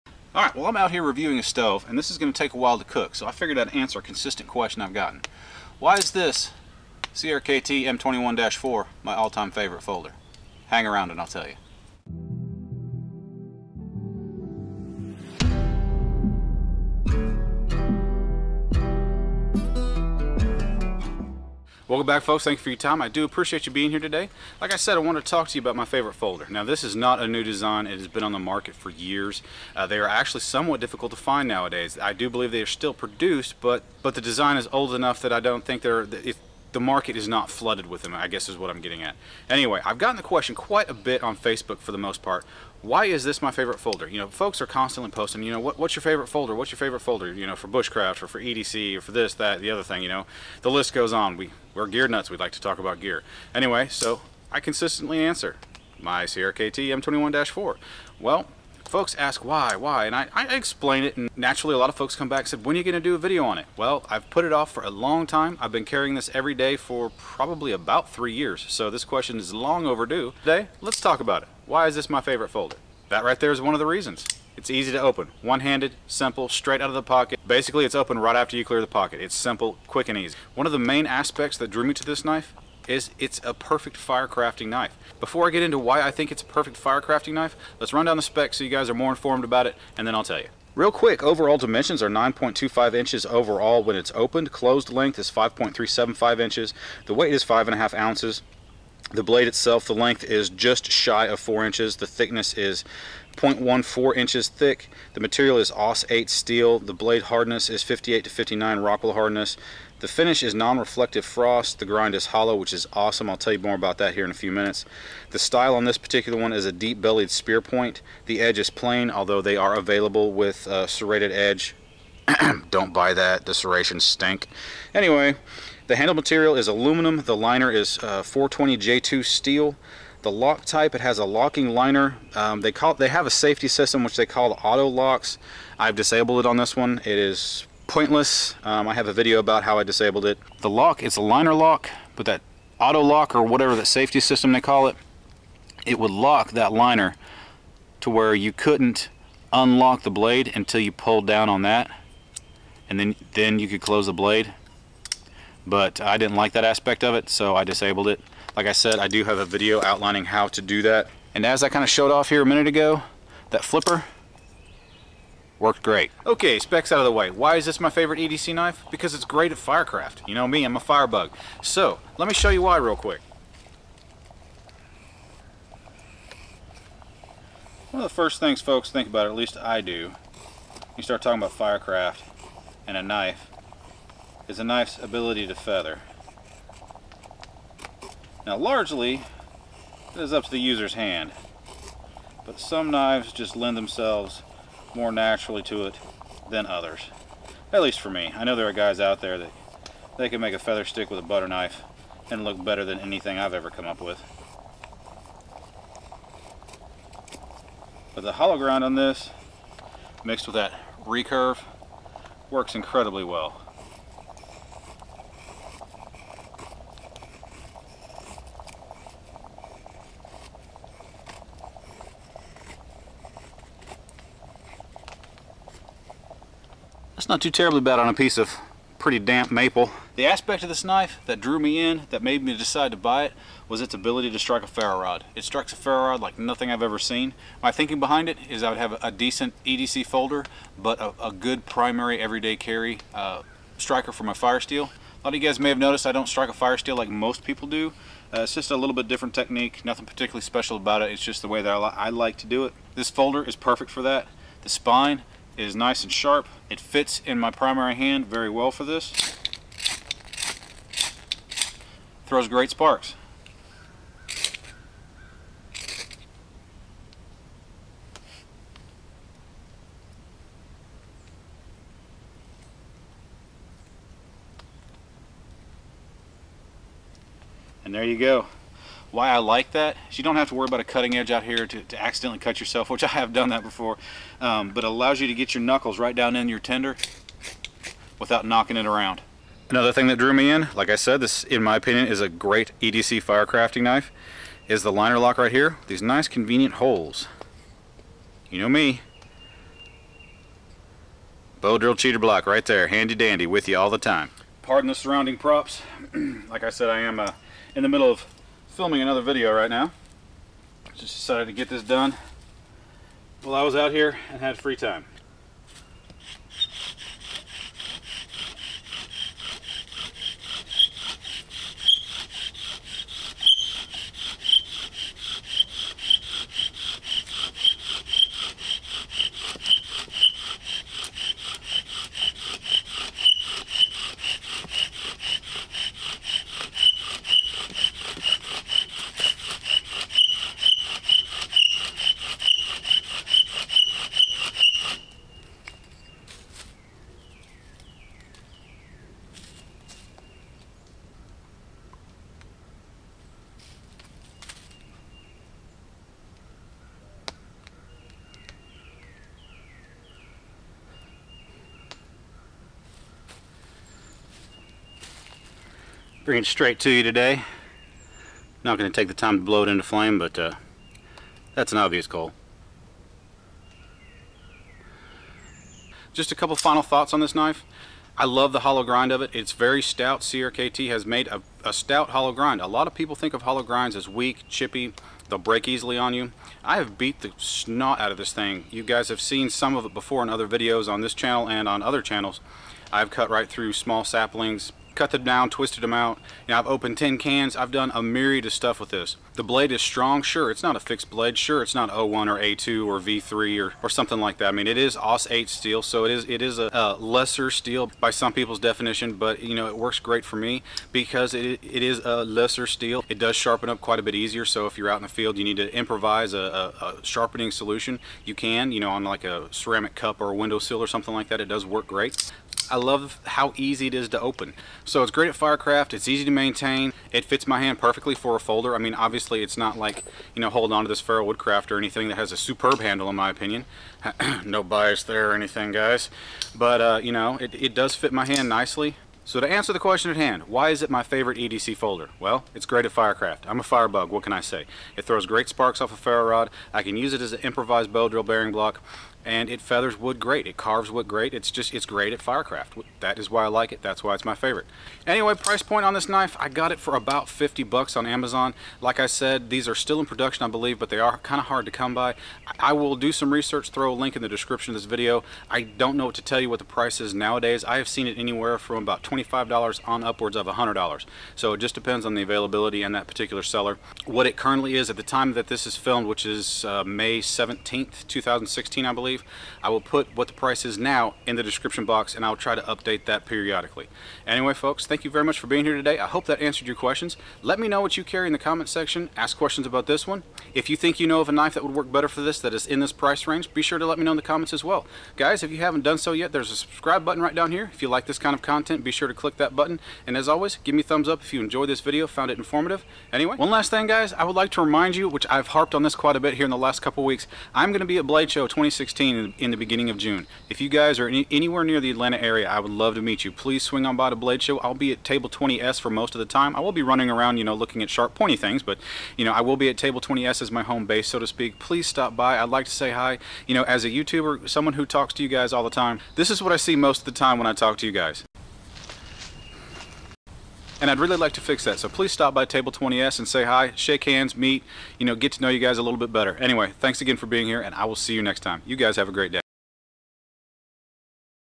Best EDC Folding Knife: CRKT M21-04
I get asked quite frequently what I think the best EDC folding knife is.  EDC knives are a very personal thing but being what it is, we all love to debate this.  Here is my personal choice for the Best EDC/Bushcraft folding knife.  It is a CRKT M21-04.  What drew me in was just how well the spine scraped sparks off a ferro rod as well as it's excellent carving profile and ability to be used as low friction bearing block for a bow drill.  In my opinion it is an excellent choice for an EDC/Bushcraft folding knife, especially if you are a fire bug like myself.

Currently this blade (slightly different model with G10 scales) is $39.95 found

Amazon Store